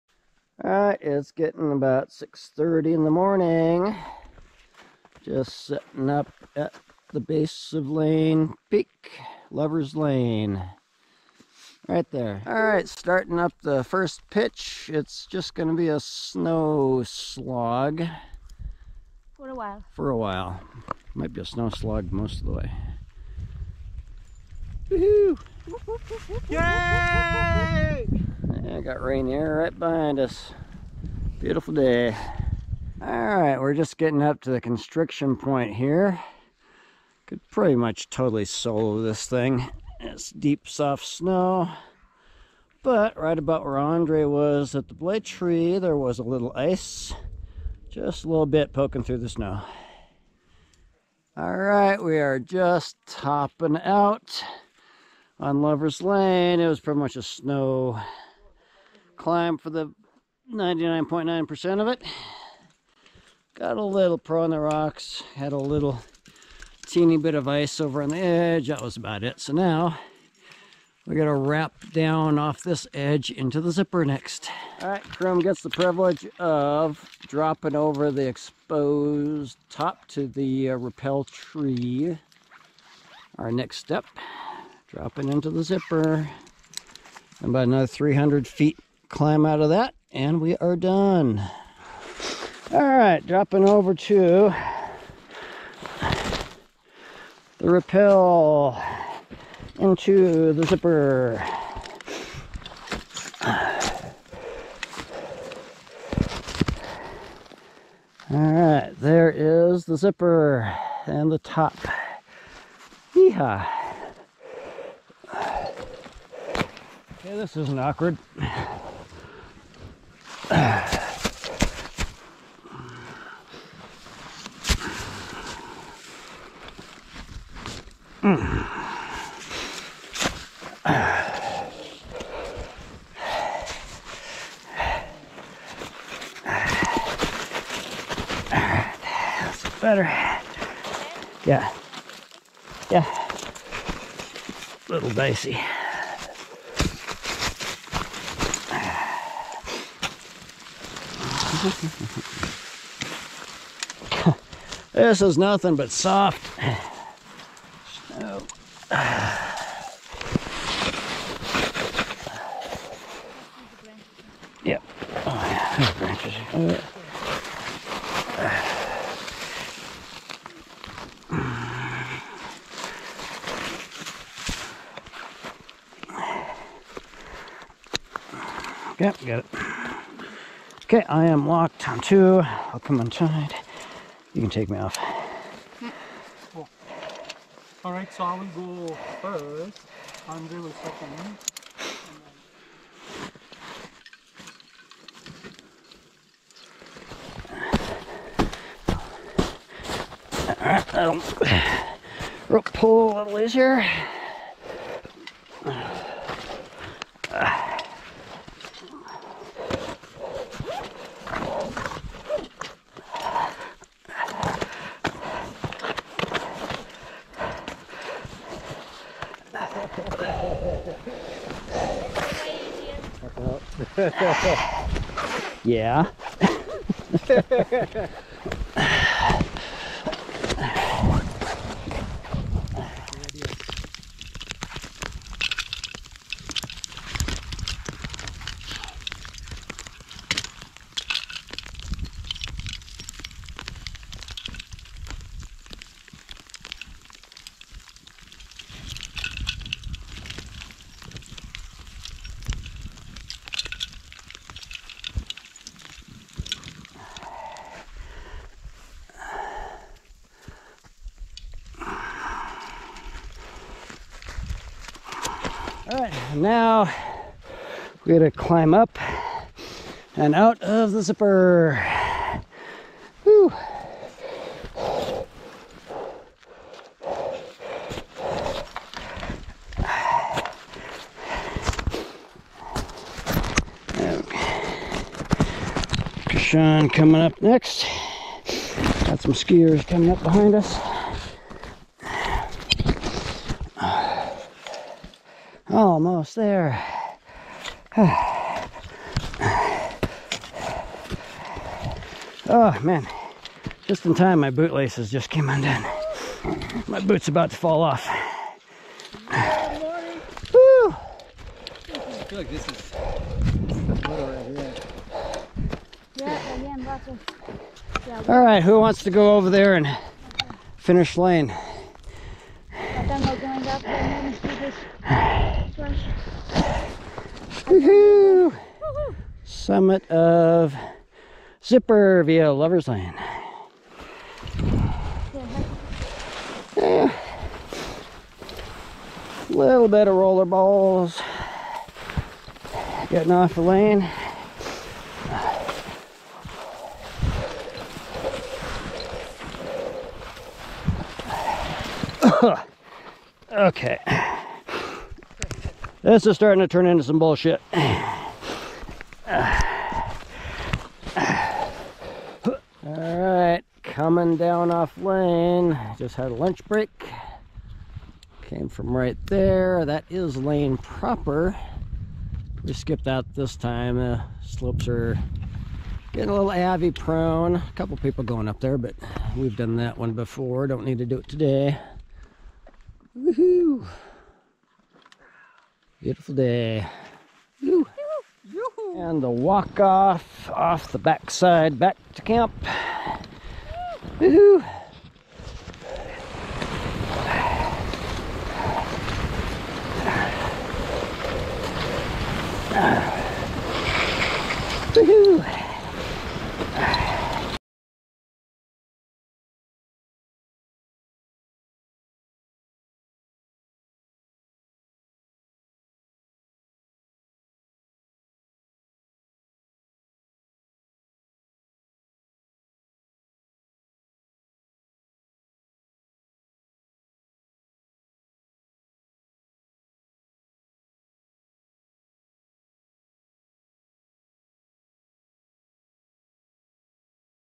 Lane Peak - Lovers Lane
Lovers Lane route on Lane Peak, rappelling into the Zipper route, climbing out of the Zipper then walking off. Steep snow climb in both routes right now.